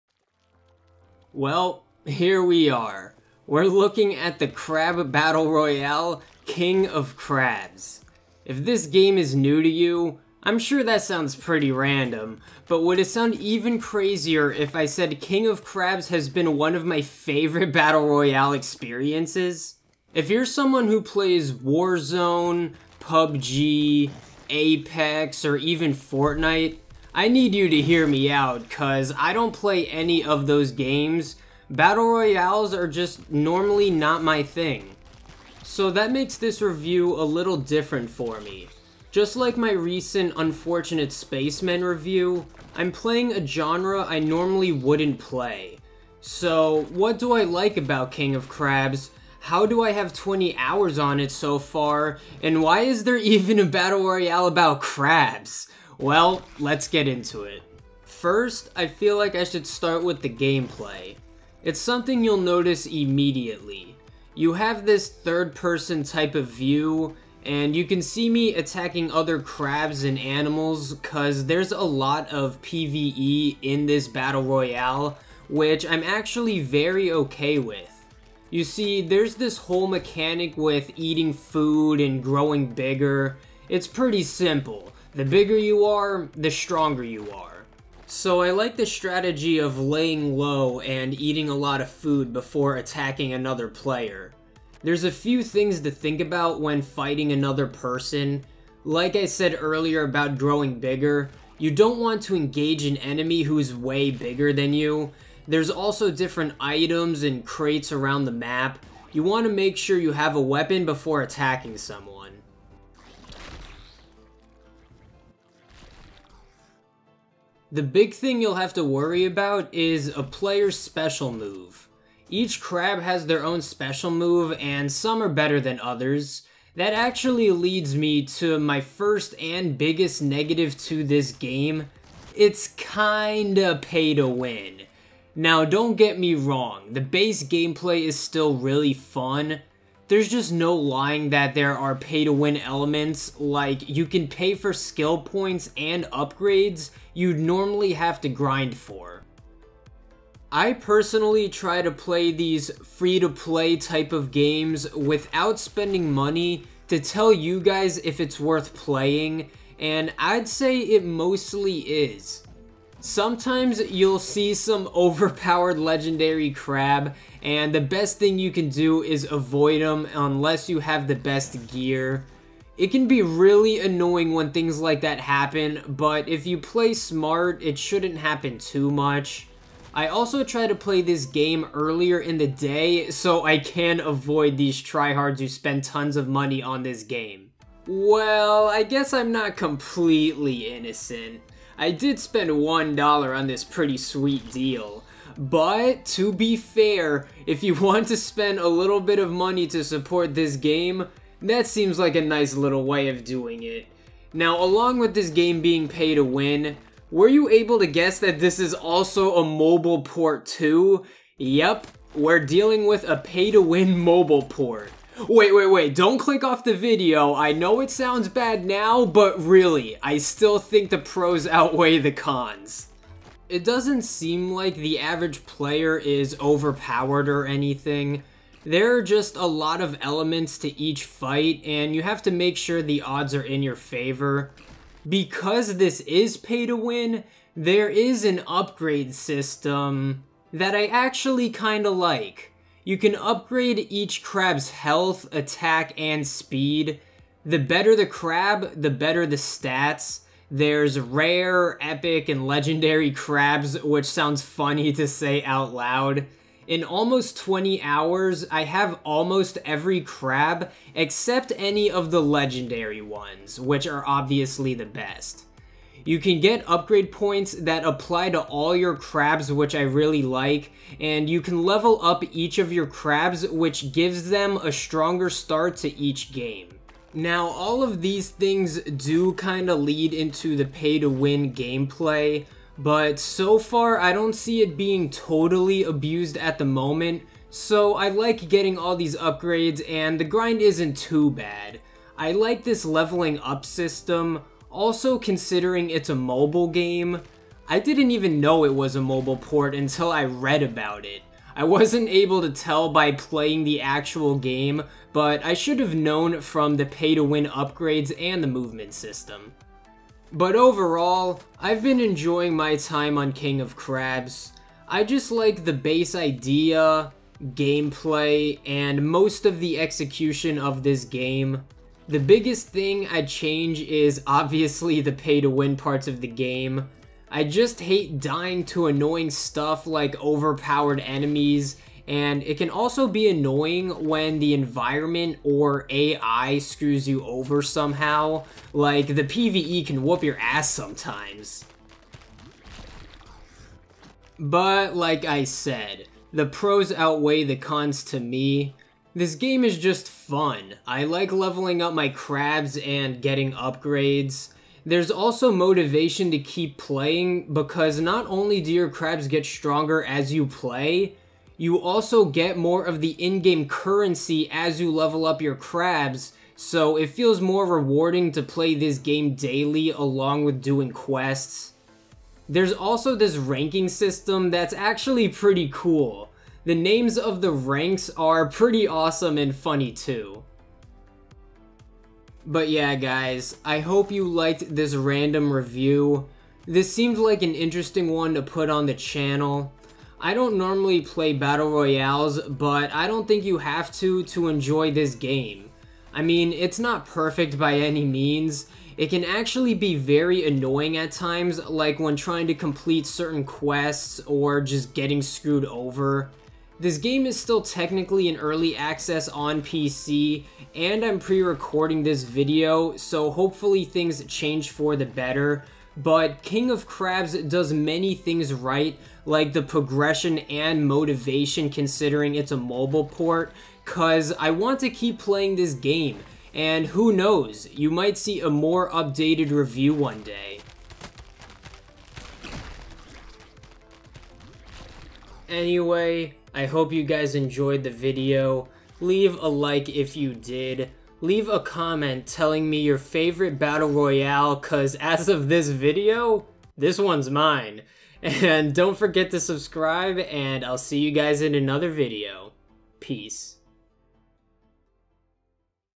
King of Crabs [The Crab Battle Royale Game Review]
Welcome to my Review for King of Crabs!
Support by rating or leaving a comment :)

A Battle Royale with crabs?! Easily Top 1 Best Games of All Time!!